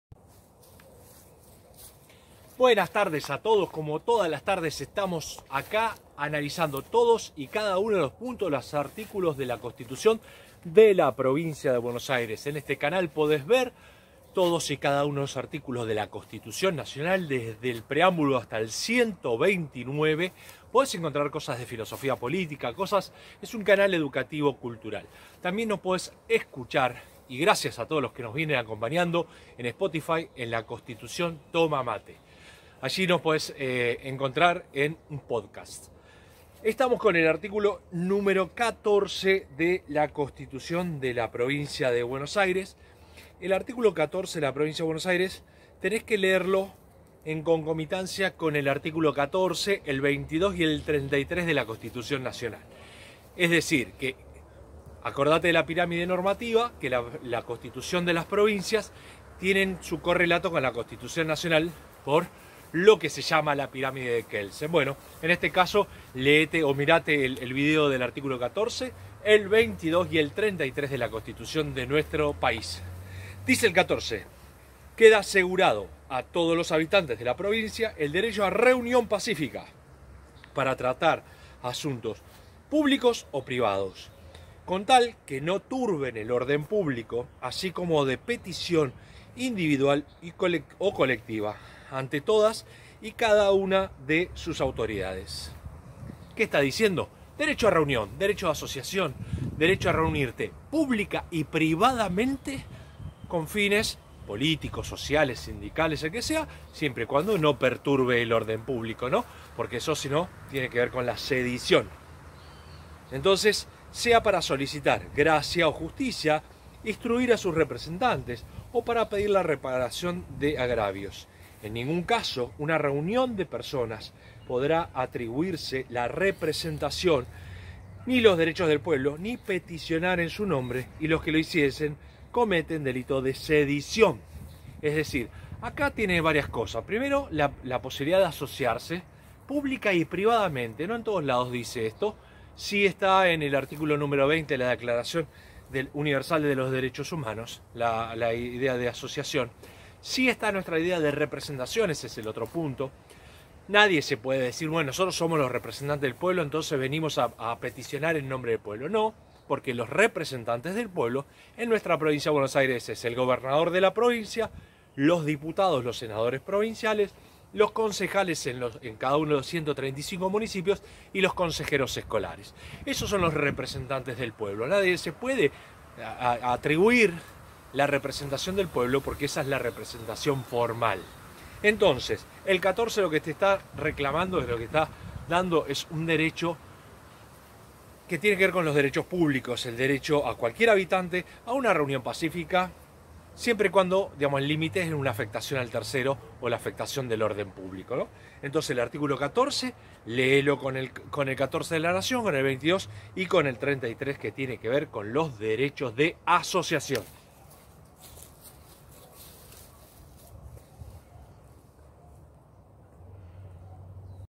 CONSTITUCIÓN DE LA PROVINCIA DE BUENOS AIRES. - Art. 13 - Gustavo Damián Gonzalez
Análisis del Art. 1 de la Constitución de la provincia de Bs. As.

En este video analizaremos el Articulo 13 de la Constitución Nacional Argentina.

CONSTITUCIÓN BUENOS AIRES -  Artículo 13 - Gustavo Damián Gonzalez

Provincia de Buenos Aires. Constitución provincial

Análisis del Art. 13 de la Constitución de la Provincia de Buenos Aires.